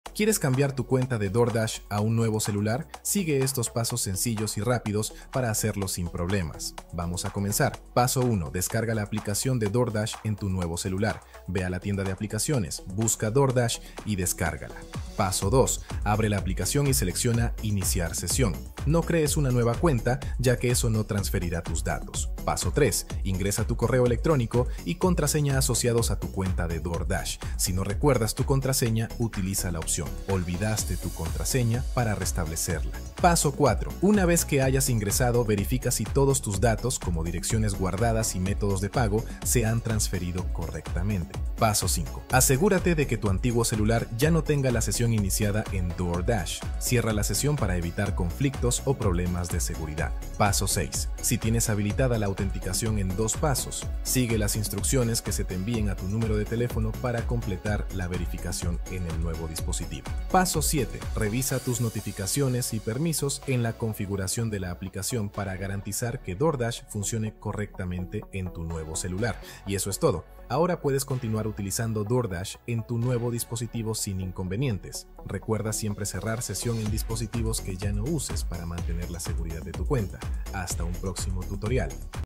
Como CAMBIAR CUENTA De DOORDASH A OTRO Celular (En 1 Minuto!!)
En este video te mostramos cómo cambiar tu cuenta de DoorDash a otro celular en solo 1 minuto. Si necesitas transferir tu cuenta, ¡este video es para ti!

-COMO-